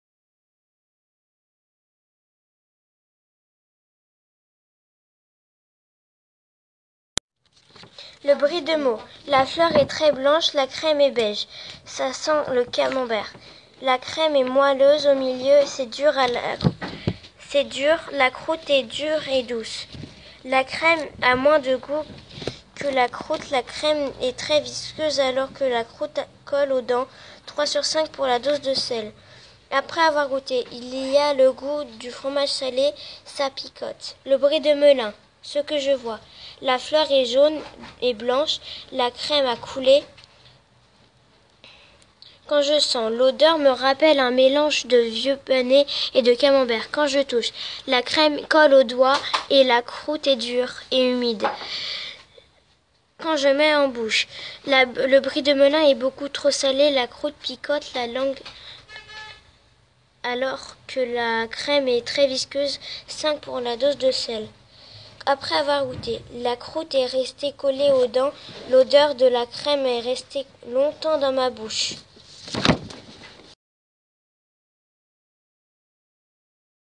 Laura a goûté et approuvé le fromage de brie !
Dans le cadre de notre classe du goût, les élèves de la classe ont produit des textes, des vidéos à propos de leurs dégustations de fromage : le brie.